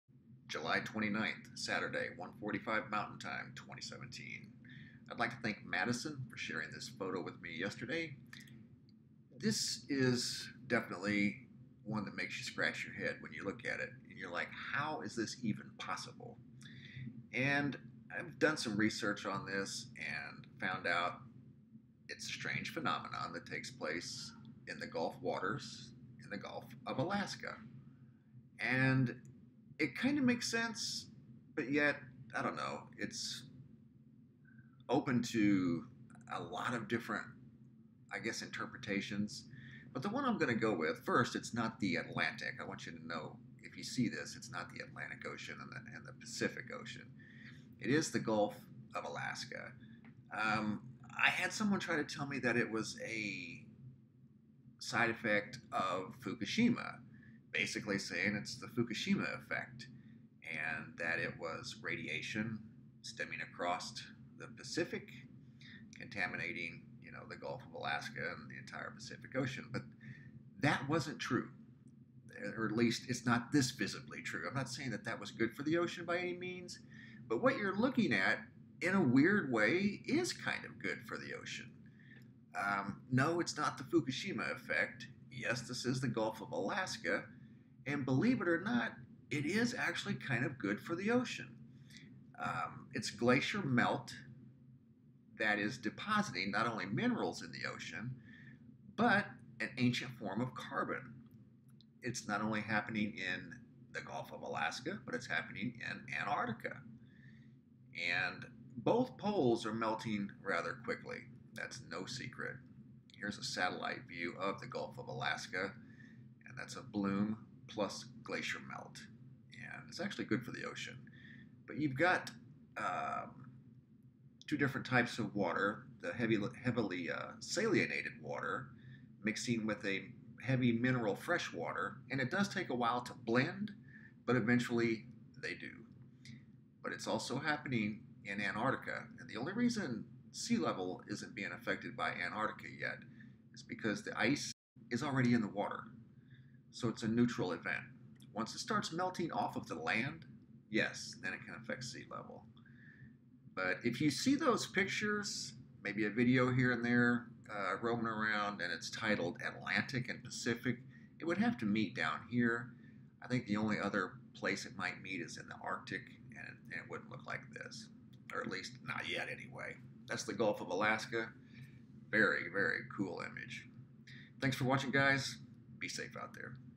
"Ocean Anomaly" | Strange phenomenon near Gulf of Alaska
July 29, 2017: Another example of the great diversity and the complex mechanics of planet earth. Probably applicable to any and all planets that thrive of H2O.